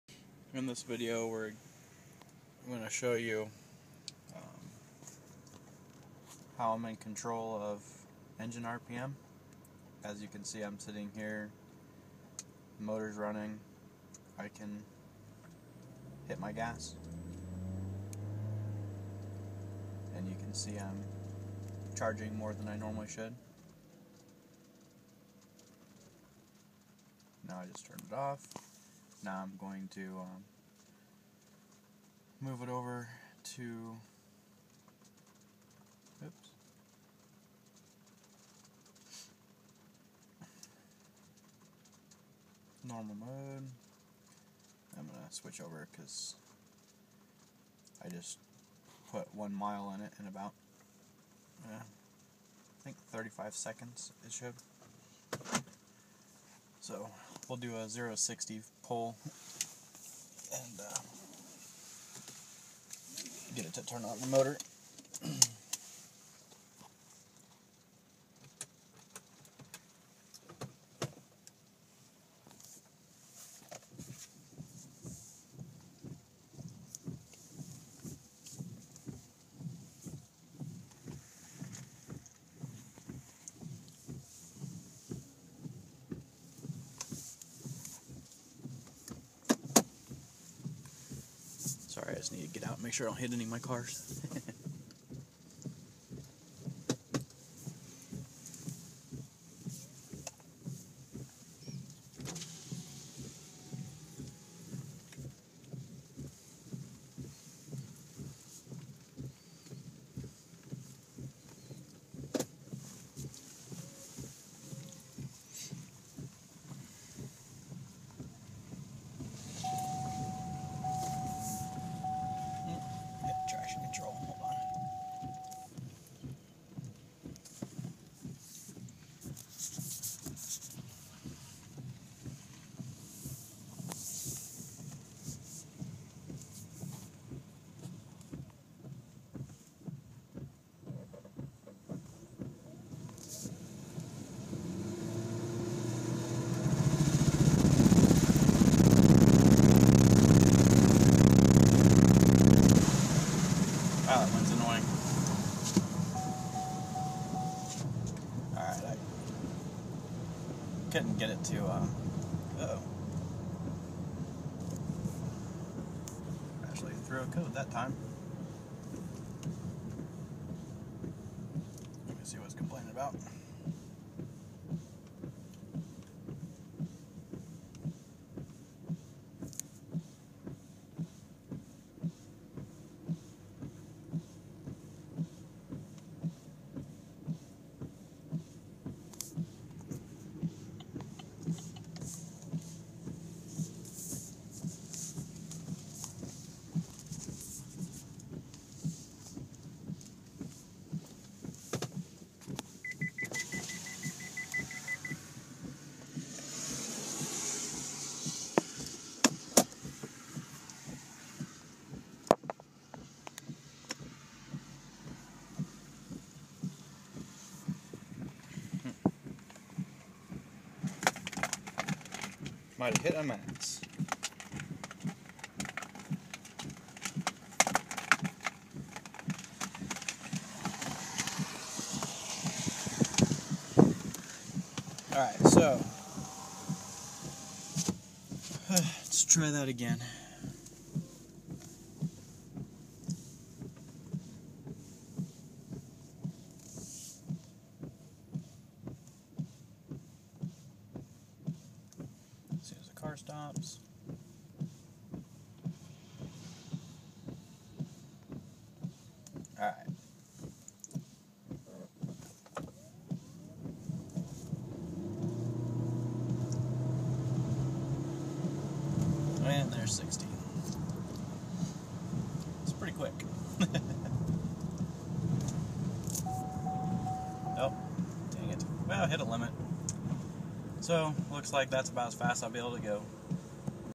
2013 chevy volt high performance part 3 fastest ever 5.5 second 0 to 60mph.MOV
Here is showing misc stuff including a few runs from 0 to 60 in under 6 seconds.   About 5.5 to be exact from the looks of the logs.  However,  it seems there is another limiter as when you do it, it goes into a reduced prop mode, and throws a code for invalid limits.

So, looks like it will have to be limited to 6 + 0 to 60s times.   Im looking for the limiter and hopefully in a few days will be faster still.  ;)